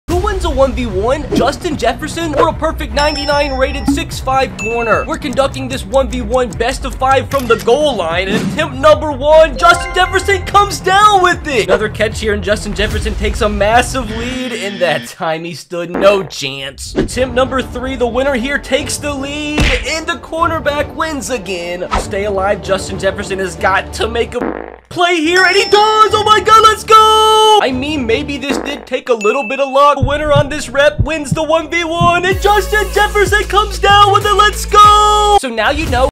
Justin Jefferson Vs. PERFECT 99 Overall Cornerback
Who wins a 1v1, Justin Jefferson or a 99 Rated Cornerback with PERFECT Ratings?